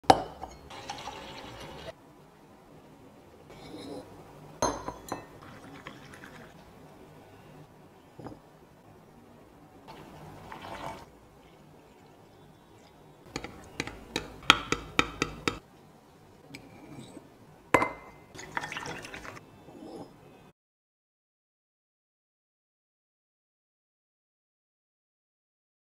Double Wall Glass Mugs with Handle | Best Coffee & Tea Mugs | Borosil
Coffee never tasted better! Enjoy your morning brew in style with Borosil Milano Double Wall Glass Tumbler. Keeps your coffee hot, your hands cool, and your mornings effortless. Make every sip count with Borosil Milano Double Wall Glass Tumbler.